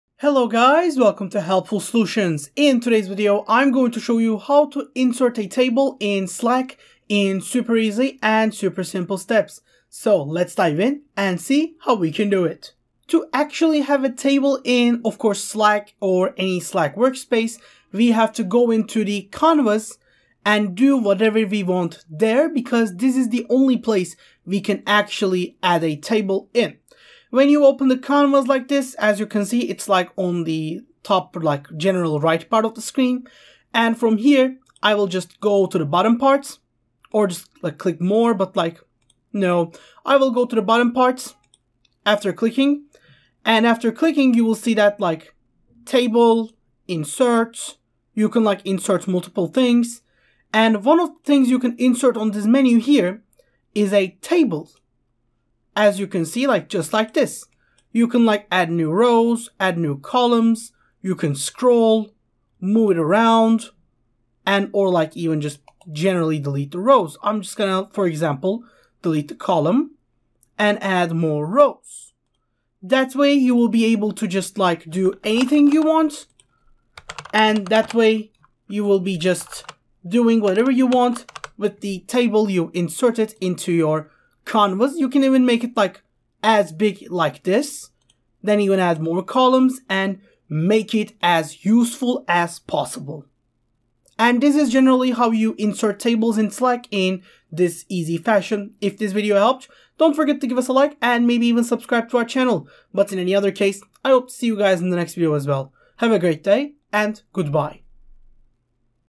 How To Insert Table In Slack (How To Add Table In Slack)
How To Insert Table In Slack (How To Add Table In Slack). In this video tutorial I will show how to insert a Table in Slack.